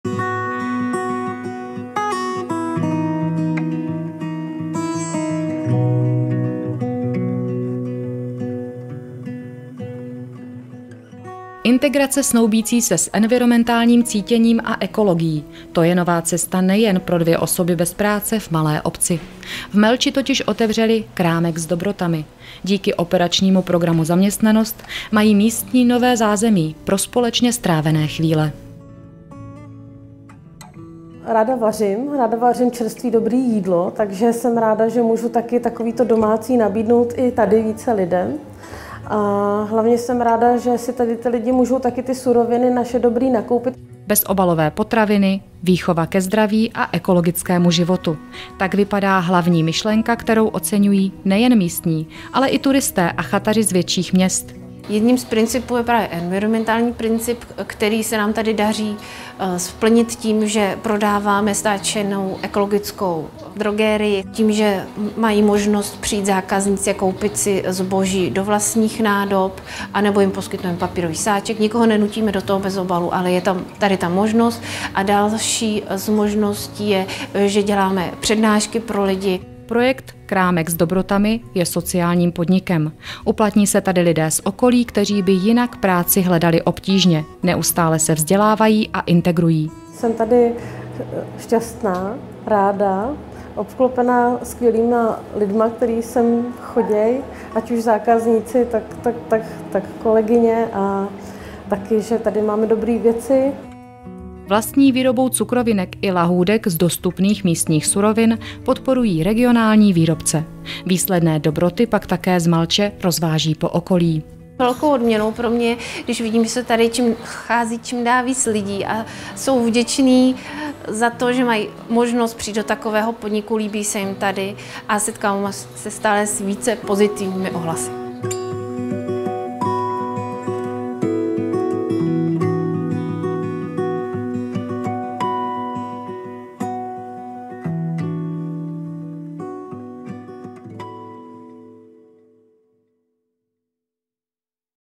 Krámek s DOBROtami
Krámek s DOBROtami je integrační sociální podnik, ve kterém byla vytvořena dvě pracovní místa. Podnik funguje jako obchod s širokým, inovativním sortimentem, nabízí občerstvení a posezení pro místní i turisty a má i svoji vlastní výrobnu (cukrářský a lahůdkářský sortiment s rozvozem). Podporuje regionálních výrobce, chráněné dílny a zdravé potraviny.